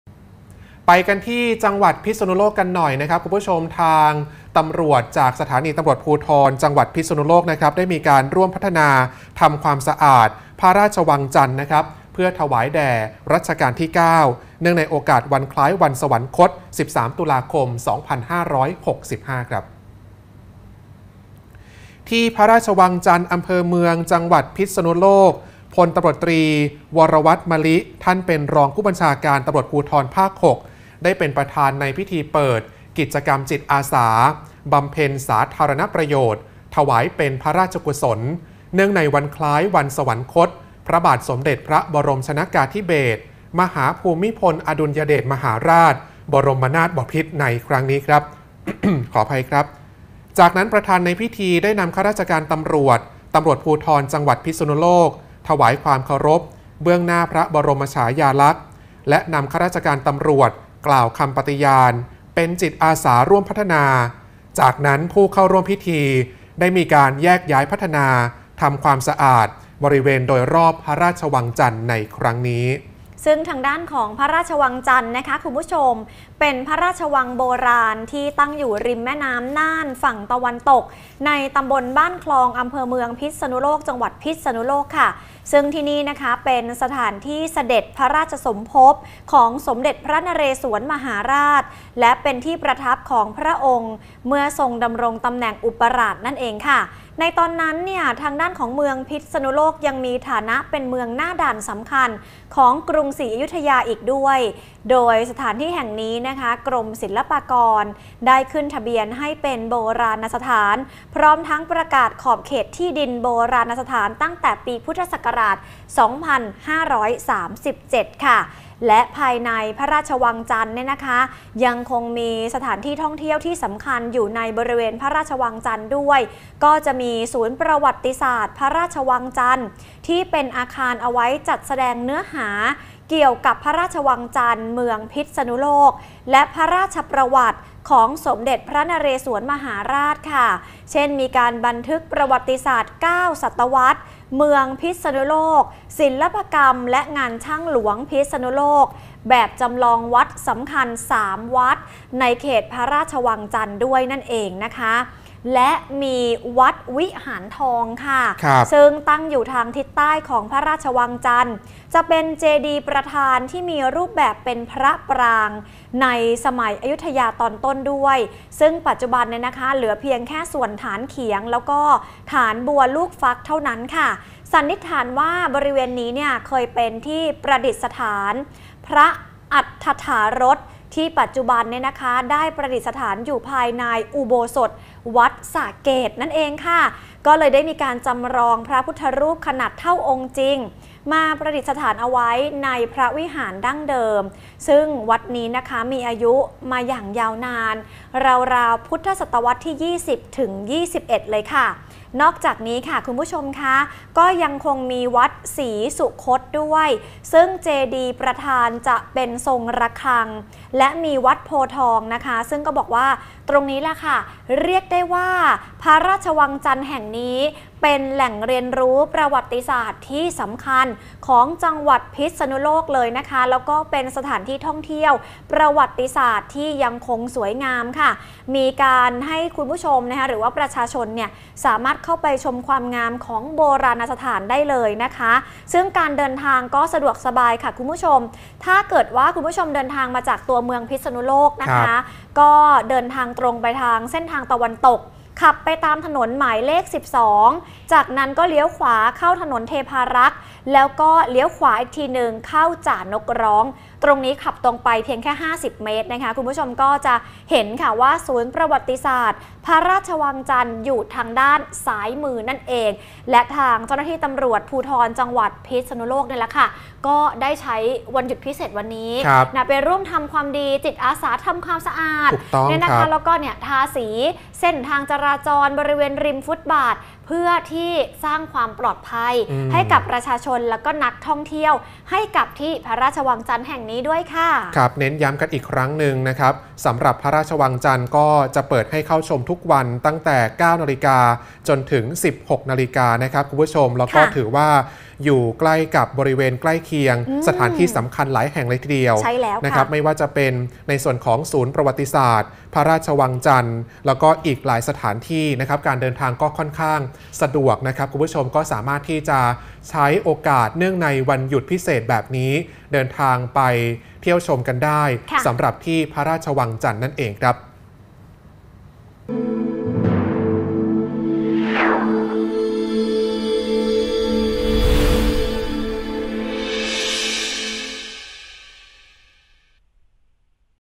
ตร.พิษณุโลก พัฒนาพระราชวังจันทน์ ถวายแด่ในหลวงรัชกาลที่ 9
ตำรวจภูธรจังหวัดพิษณุโลกร่วมพัฒนาทำความสะอาดพระราชวังจันทน์ ถวายแด่ในหลวงรัชกาลที่ 9 เนื่องในโอกาสวันคล้าย วันสวรรคต 13 ตุลาคม